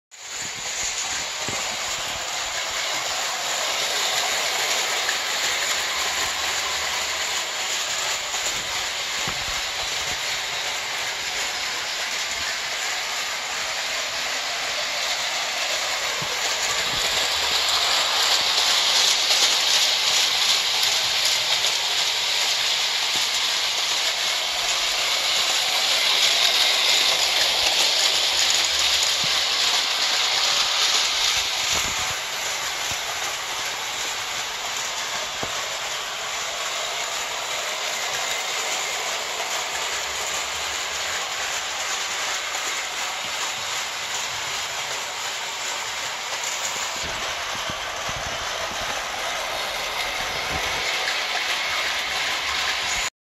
German Marklin 1 gauge American market NYC passenger steam train
1912 Marklin American style steam electric toy train